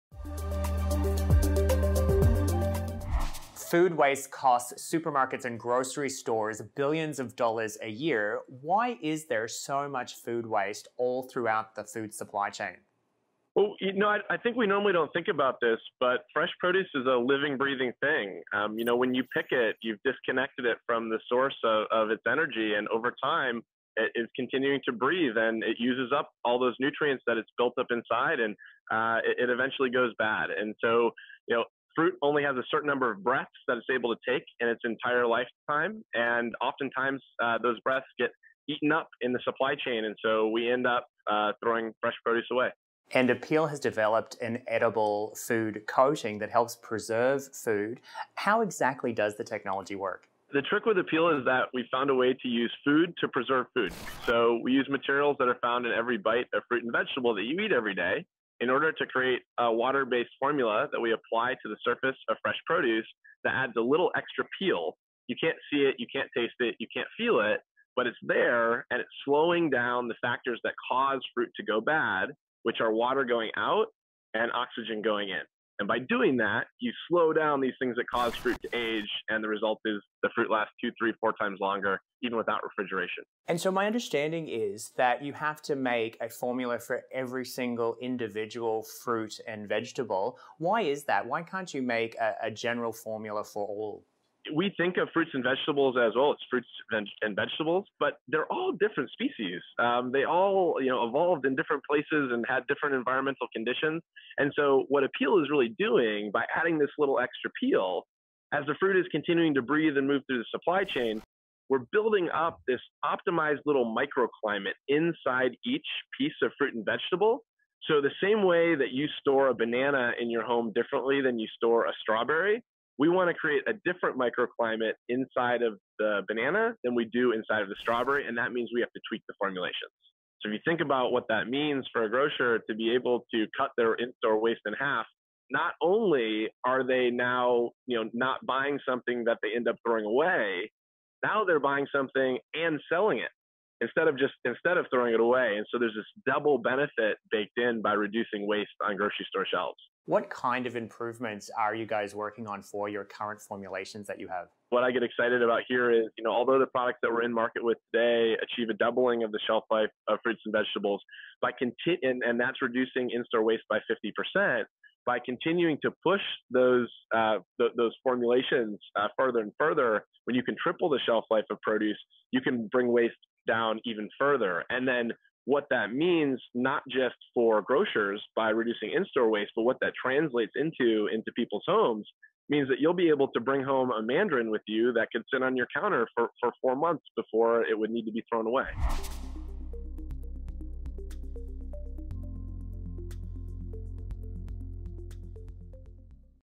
Beyond Innovation S2: James Rogers, Apeel Sciences
The food supply chain is littered with waste at nearly every stage, often with fresh produce thrown away before it even reaches the grocery store. Water based formula is tasteless, odorless, and invisible to the touch, but will keep fruit ripe 3-4 times longer. Founder and CEO of Apeel Sciences James Rogers tells Michael Bancroft how his company is reducing waste, while also saving grocers money.